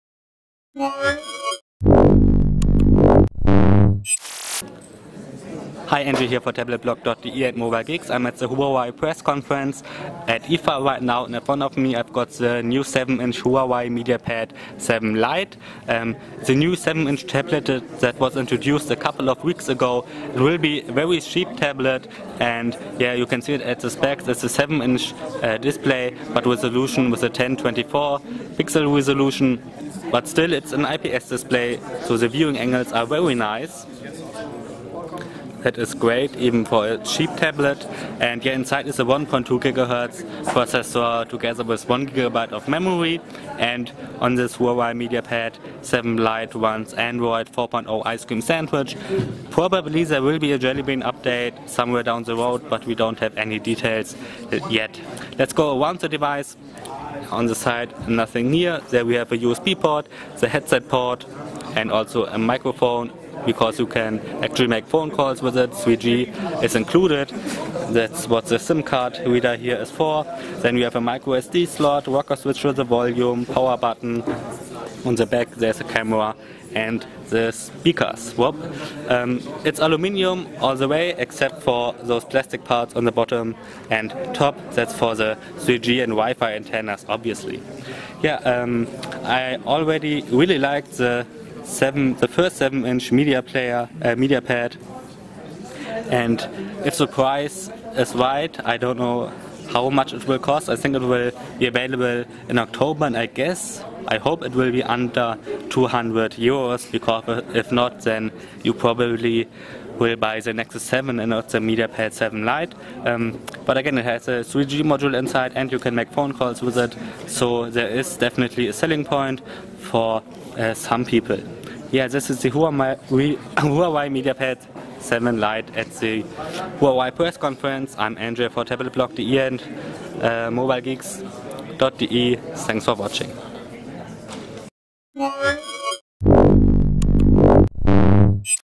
Huawei MediaPad 7 Lite Hands On at IFA 2012
Huawei MediaPad 7 Lite Hands - Huawei MediaPad 7 Lite Hands On at IFA 2012 - We take a look at the cheap Huawei MediaPad 7 Lite with a 7inch display, 1.2GHz SingleCore CPU and Android 4.0 Ice Cream Sandwich